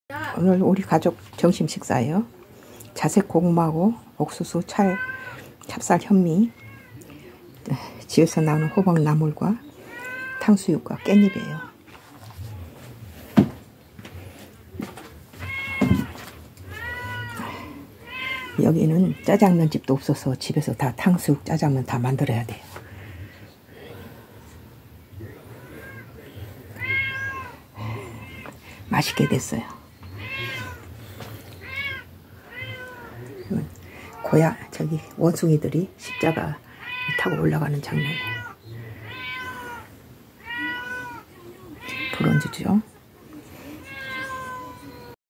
MAMA FOOD Mom’s vote! Plate food~~~!
Mom's vote! Plate food~~~!

There are still a lot of sweet potatoes and pumpkins that I couldn't dig up due to farming! There are still a lot of them in the fields!
There is no time to spend leisurely~~~!
Today’s lunch~~! Put the food you want to eat on your plate!
I decided to eat something simple~~~!

Purple sweet potato meat sweet and sour pork~~I can eat it all the time and it’s amazing~~!
Pumpkin greens picked from the field and perilla leaves as a side dish whet your appetite!